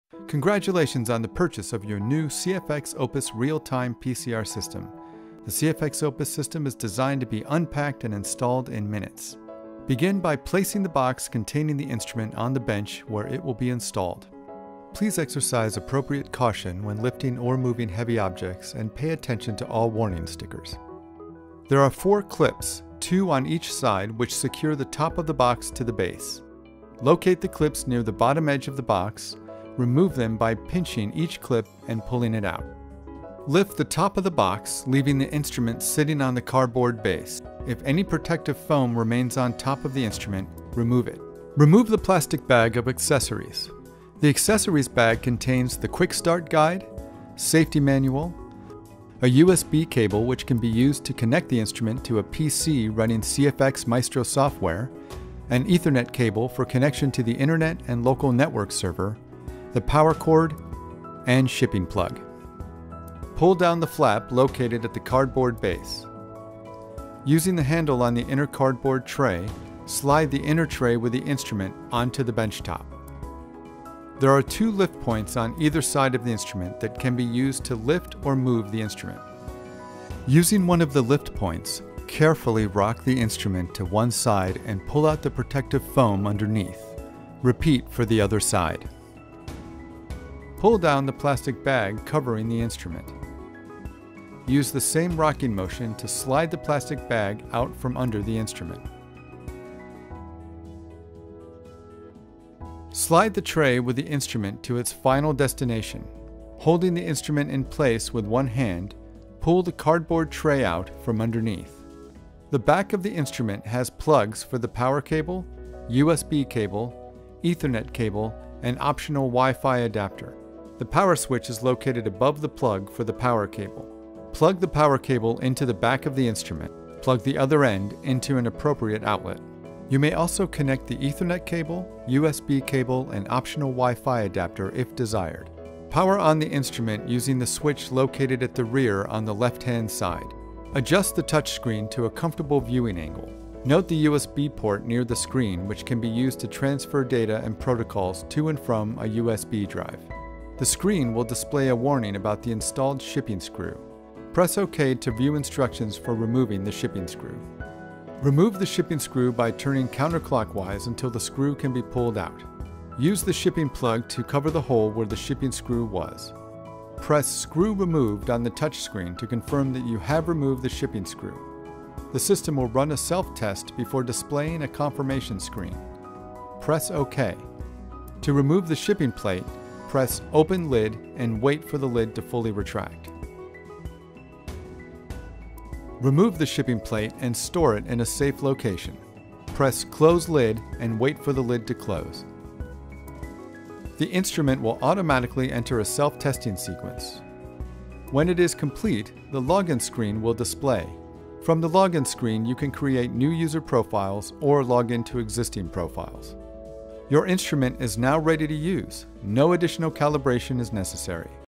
BioRad CFX Opus Real Time PCR System | 96 well | 384 well | Unboxing | Installation Guide
The CFX Opus system is the next evolution in qPCR from Bio-Rad. With improved thermal performance and our proprietary, accurate optical shuttle system, your data will be more consistent than ever. A sleek, modern design includes a refreshed and easy-to-use interface with new connectivity options and features.

In addition to ethernet compatibility, the CFX Opus system can connect directly to wi-fi for completely wireless operation and data retrieval. The CFX Opus system is capable of stand-alone operation and direct data transfer to a USB device, or it can be connected to a PC.

With internet connectivity, the CFX Opus system can be programmed to send your data and notifications directly to your email or local networked drive, enabling easy access by collaborators or from other network-connected locations.

Features:
- Set up your system quickly — easy installation and factory-calibrated optics
- Our best thermal uniformity and accuracy yet
- Wi-fi connectivity and BR.io cloud platform analysis tools
- Sensitive modern touch screen — responsive even with double gloves
- Updated interface is easier to use and more intuitive than ever
- Uniform optical system free of regular calibration requirements
- Audit and e-signature compatibility with CFX Maestro Security Edition Software
- Available in 96-well (5-color detection) and 384-well (4-color detection) formats